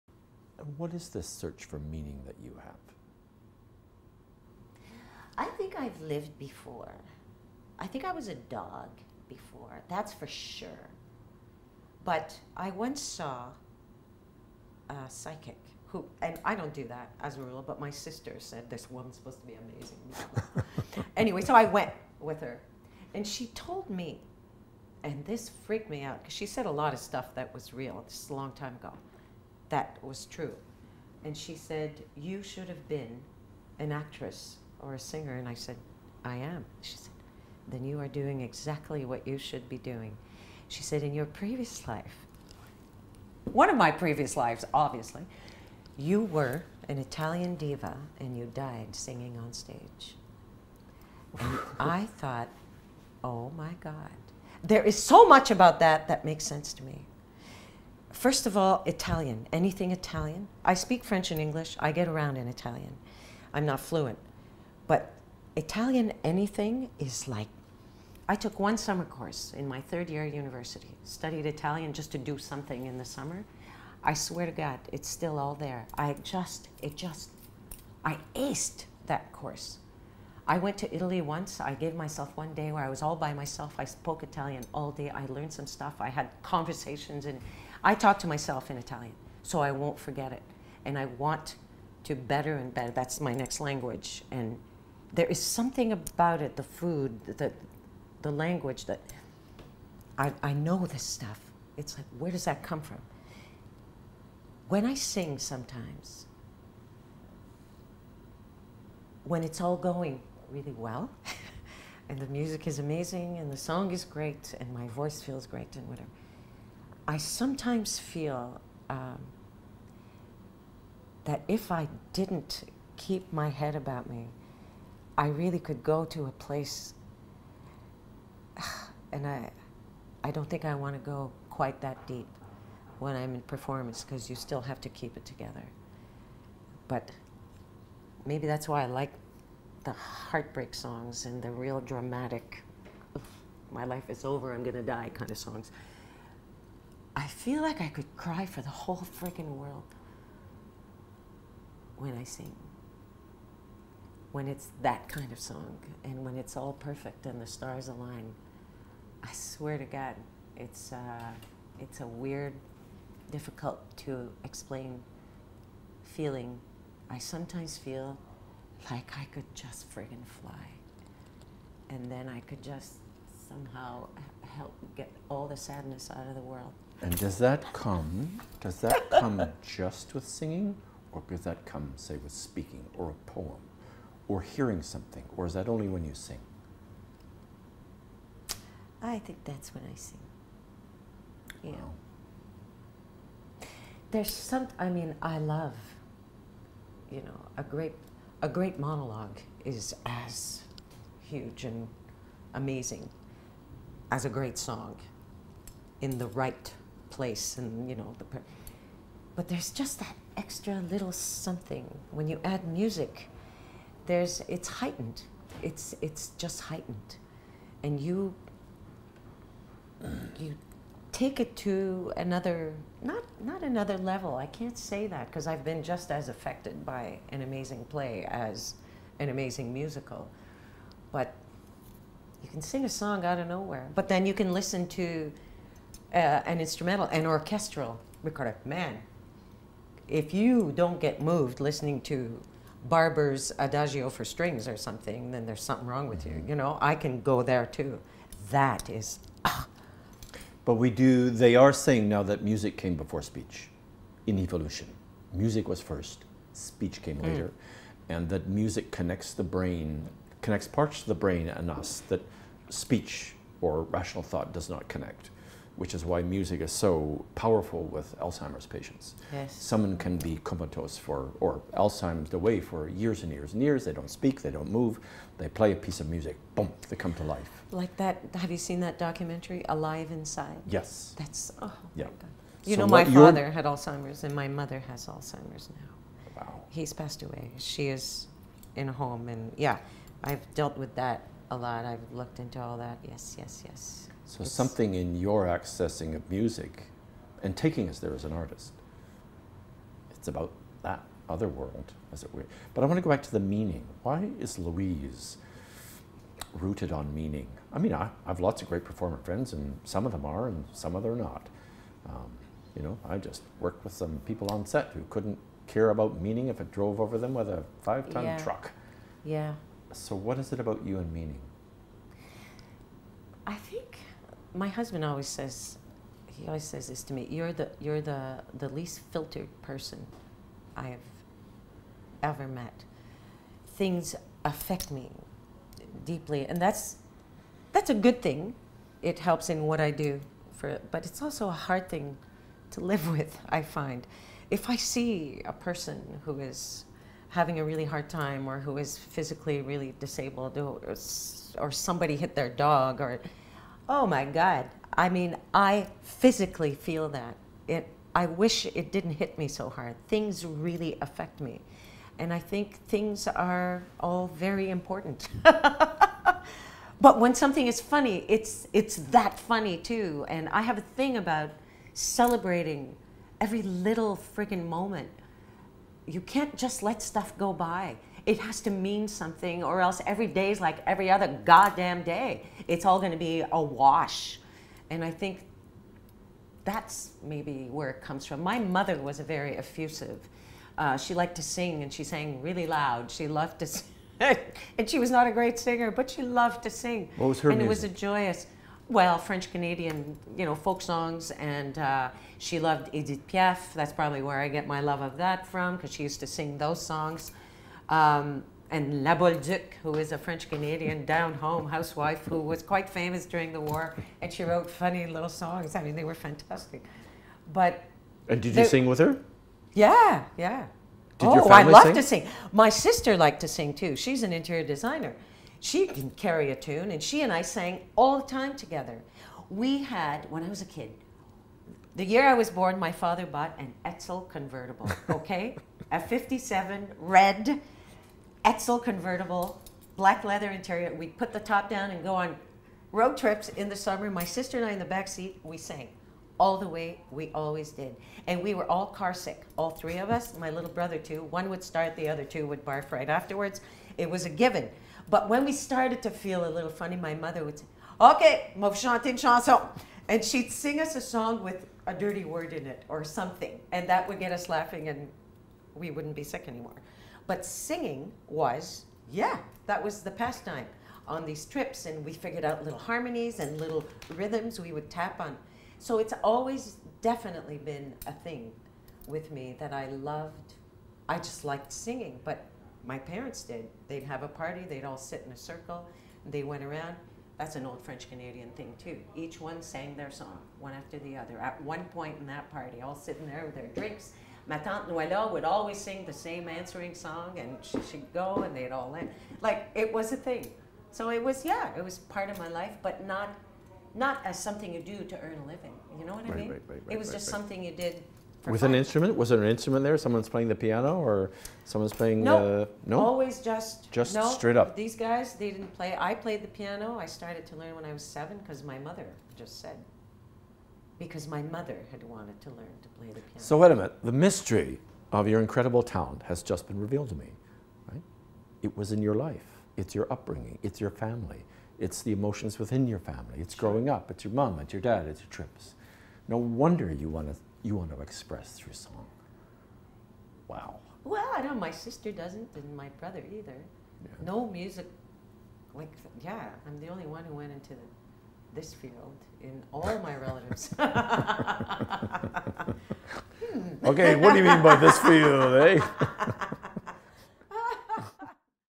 Louise Petrie on how she summons her passion
Louise Pitre discusses how she finds the passion that drives her performance. (Part 5 of 19)

Interviewed by R.H. Thomson on March 27th, 2017 in Toronto.
Filming location courtesy of Dancemakers Centre for Creation.